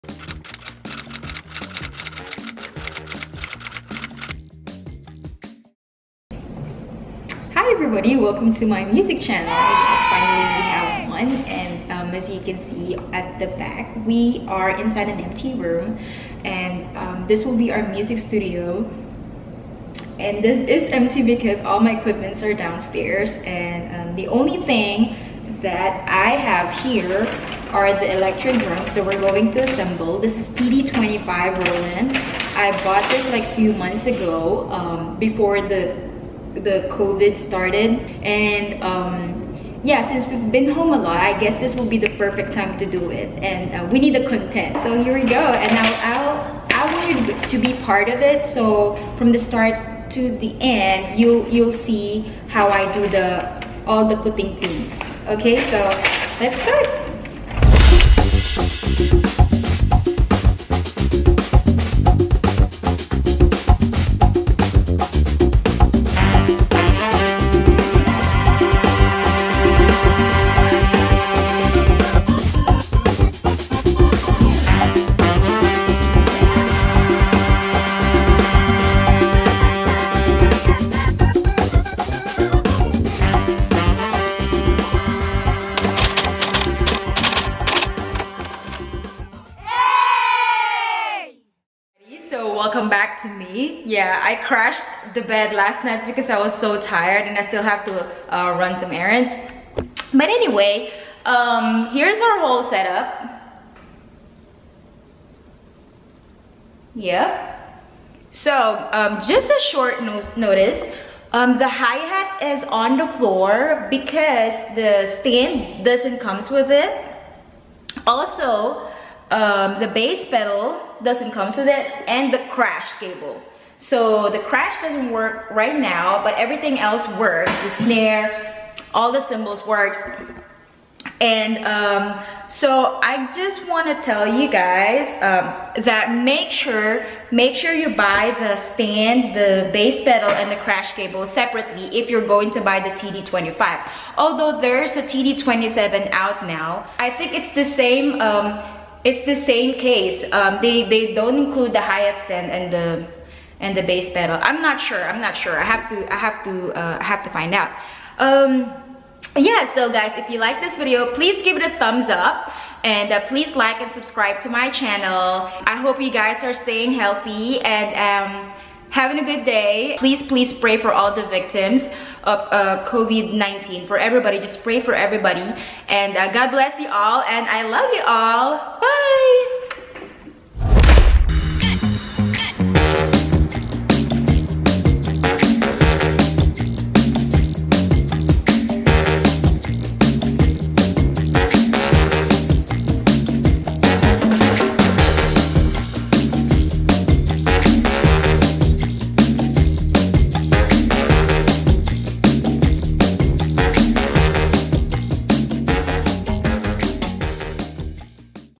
VLOG 1 | Setting up my Roland V-Drums TD-25KVX Electronic Drum Set
Just a quick video to share you all what I mostly do to keep myself busy at home during quarantine.